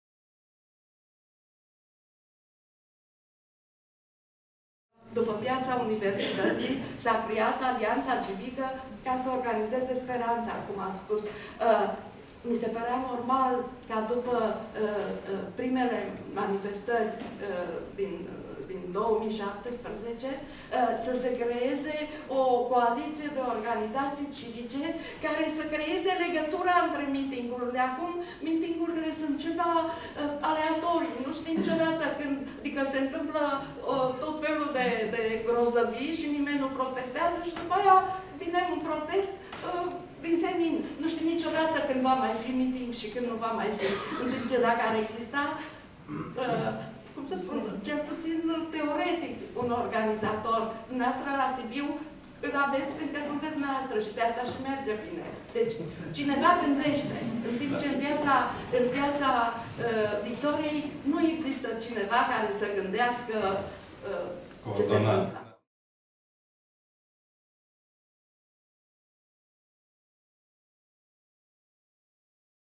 Fragment din Dezbaterea: De la cetățean la om politic. Primele partide de după '90
Participanți: Radu Vancu, Mona Muscă, Smaranda Enache, Ana Blandiana, Radu Filipescu. Moderator: Alexandru Solomon

Evenimentul a avut loc în cadrul Festivalului Internațional de Documentar și Drepturile Omului One World Romania - 15-24 martie 2019